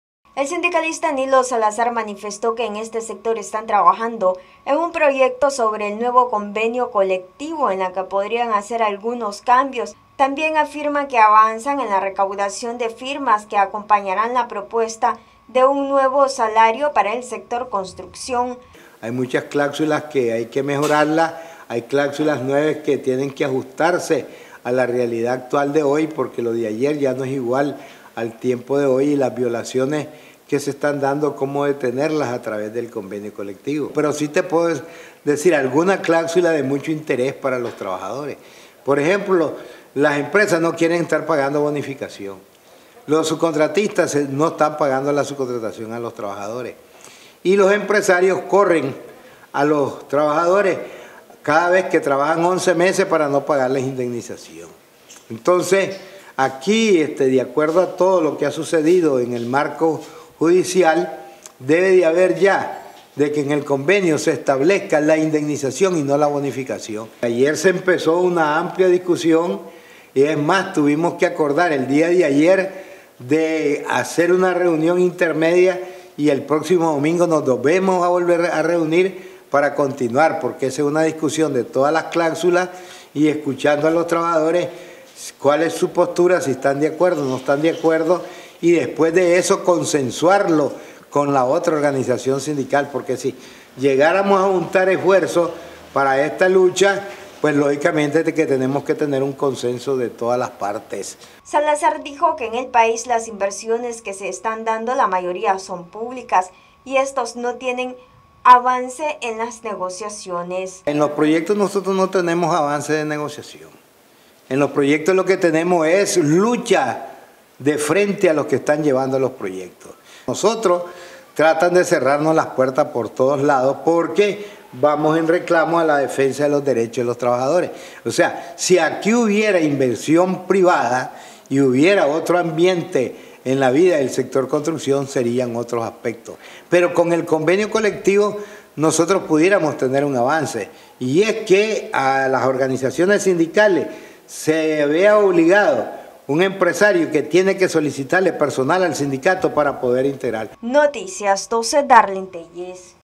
Sector construcción trabaja en la elaboración de un nuevo convenio colectivo
Noticias12Nicaragua Sindicalista del Sector Construcción manifestó que continúan trabajando en la elaboración de un nuevo convenio colectivo, así como en la recolección de unas 5 mil firmas que respalden la solicitud de un nuevo salario para dicho sector.